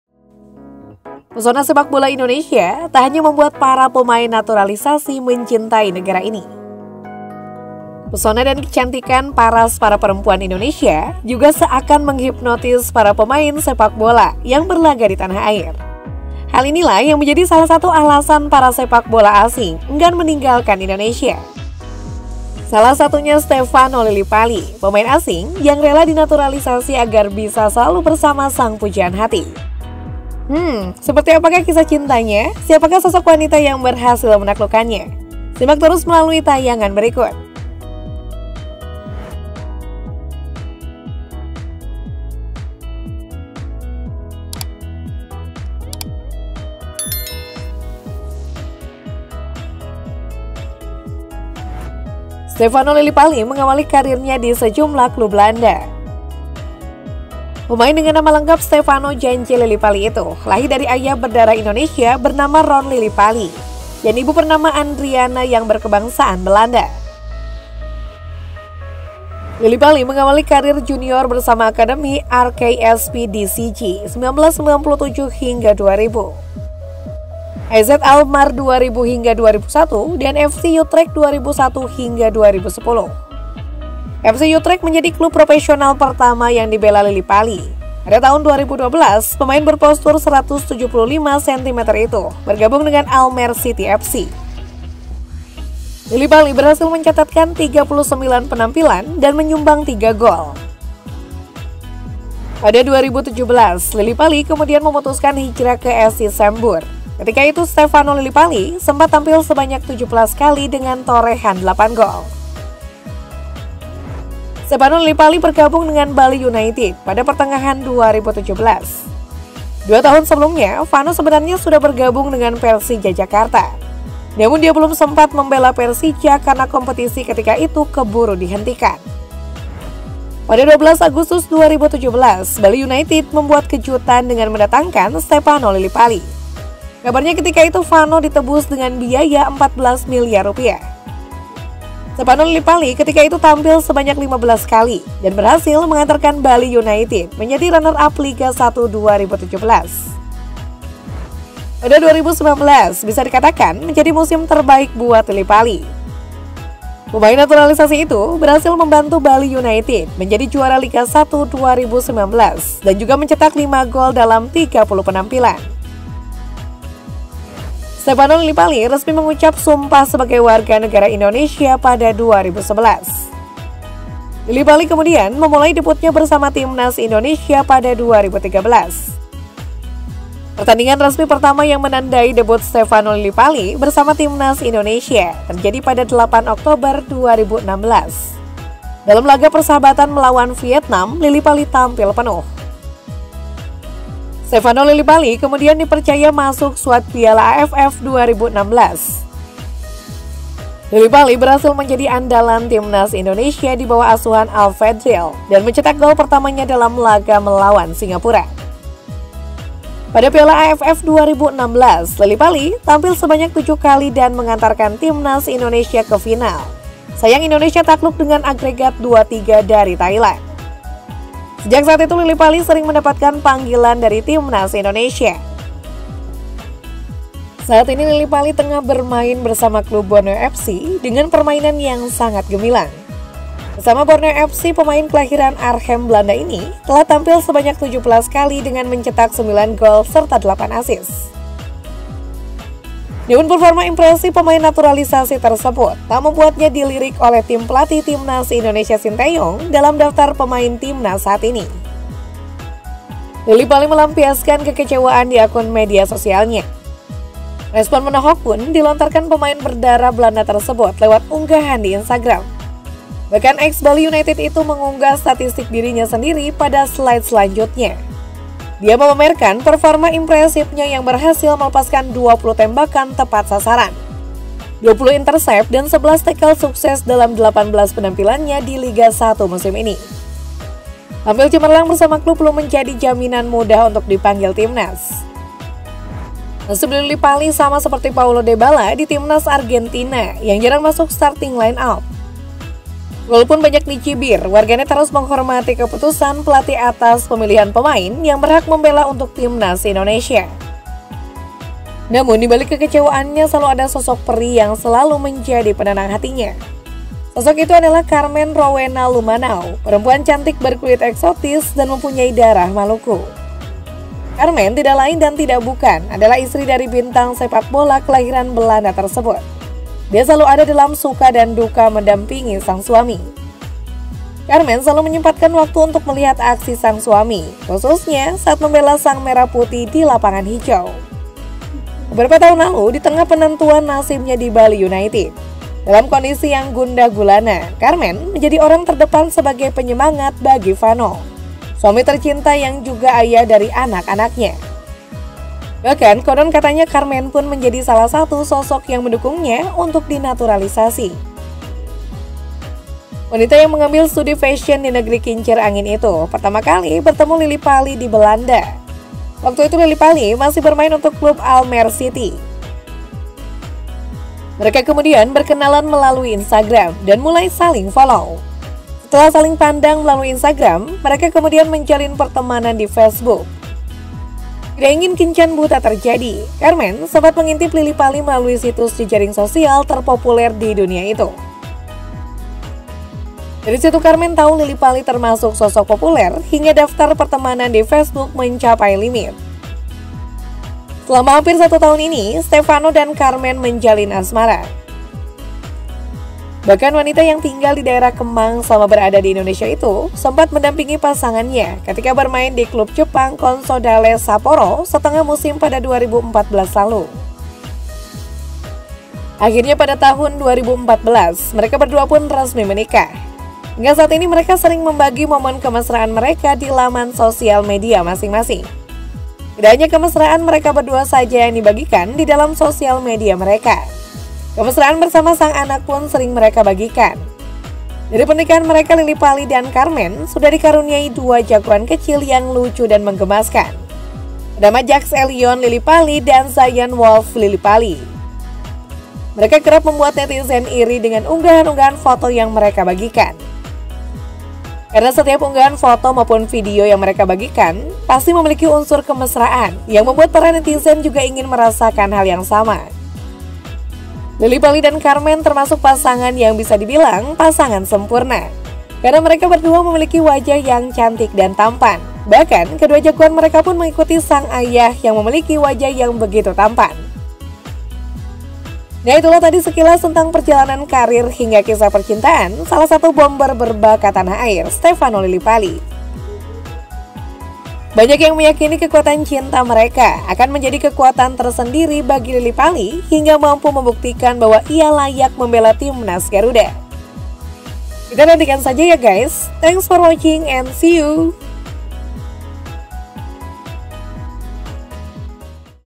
PANTAS BETAH MAIN DI INDONESIA! Ternyata Begini Kisah Cinta Stefano Lilipaly dan Carmen Rowena
Pesona sepak bola Indonesia tak hanya membuat para pemain naturalisasi mencintai negara ini, Pesona dan kecantikan paras para perempuan Indonesia juga seakan menghipnotis para pemain sepak bola yang berlaga di tanah air. Hal ini lah yang menjadi salah satu alasan para pesepak bola asing enggan meninggalkan Indonesia, Salah Satunya Stefano Lilipaly, pemain Asing yang rela di Naturalisasi agar bisa selalu bersama sang pujaan hati. Hemm, seperti apakah kisah cintanya?siapakah sosok wanita yang berhasil menaklukkannya?simak terus melalui tayangan berikut..